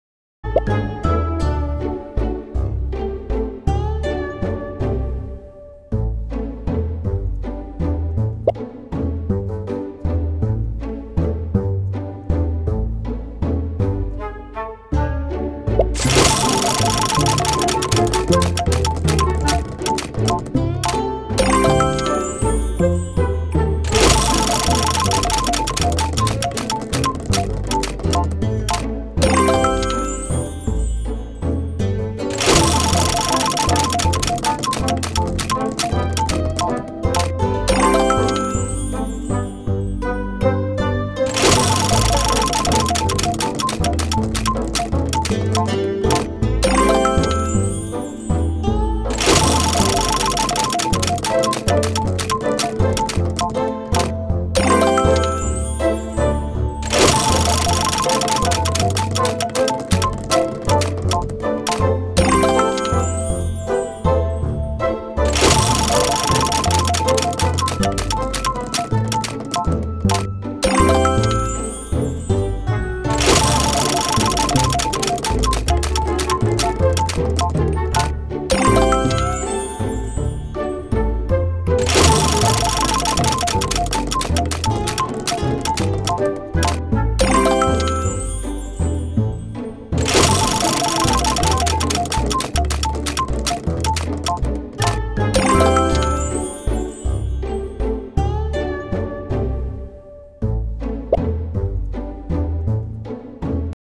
MAX Spins on The Beast Wheel
First time spinning the Mr Beast Wheel max times possible!